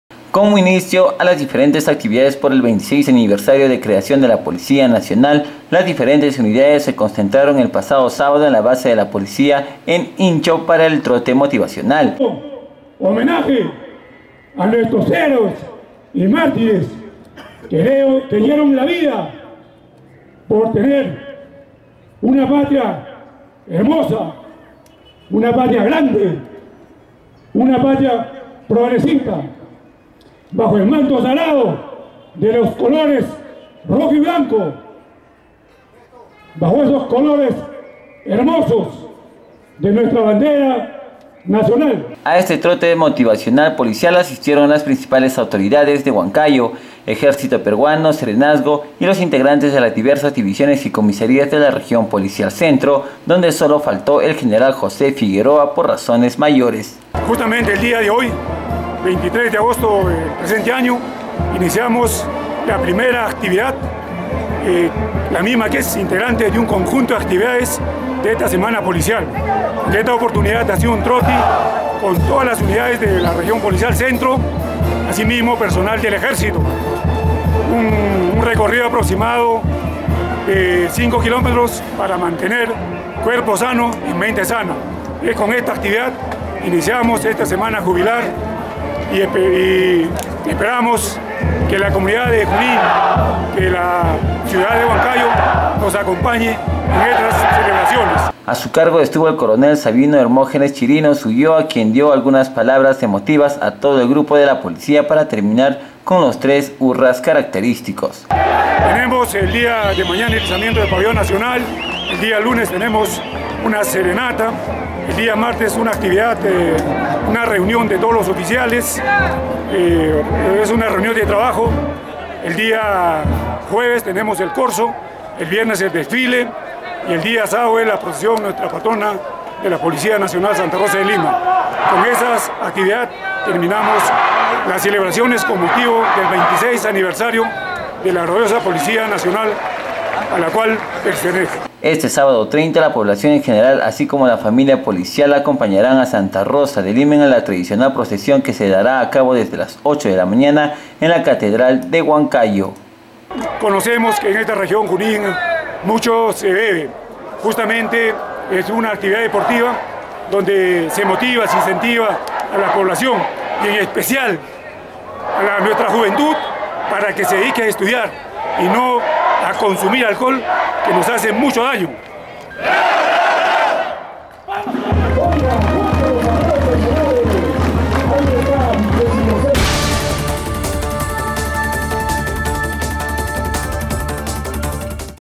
TROTE PNP
DIFERENTES ACTIVIDADES POR 26 ANIVERSARIO DE LA POLICÍA NACIONAL DEL PERU Y ALISTAN DIFERENTES ACTIVIDADES